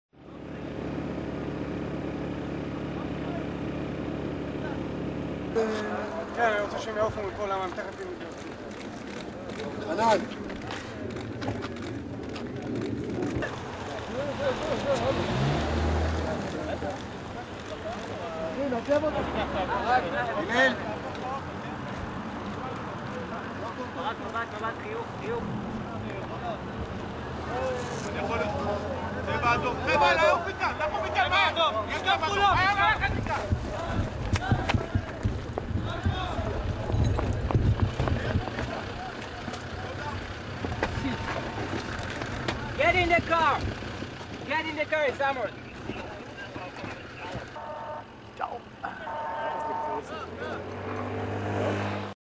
A rocket falls in Kfer Shalom
A rocket falls in Kfer Shalom in Israel